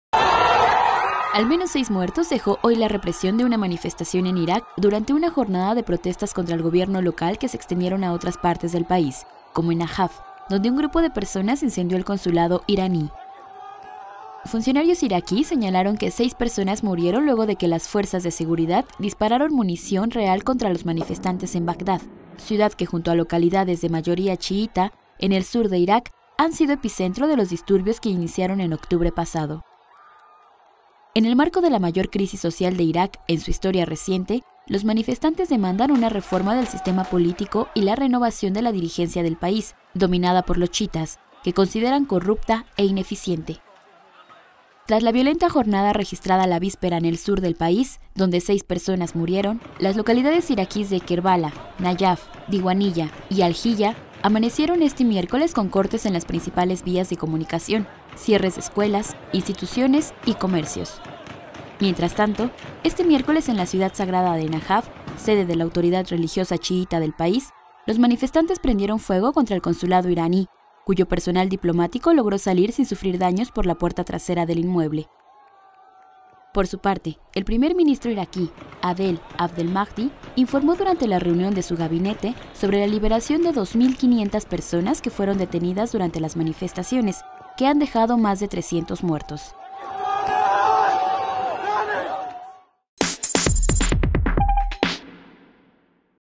Nueva jornada de protestas en Irak deja seis muertos
Al menos seis muertos dejó hoy la represión de una manifestación en Irak durante una jornada de protestas contra el gobierno local que se extendieron a otras partes del país, como en Najaf.